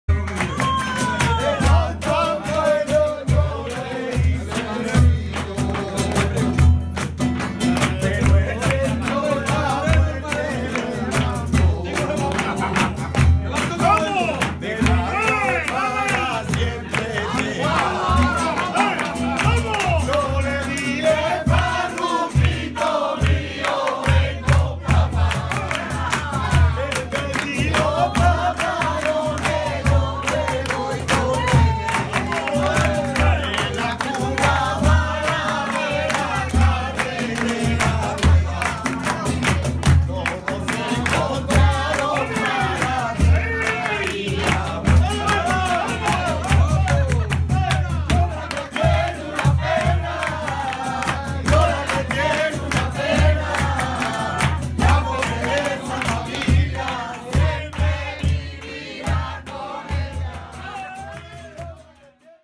Rappelkiste "El Poquito" Weihnachten 2018
Weihnachten 2018 in einer Bar in Antequera Spanien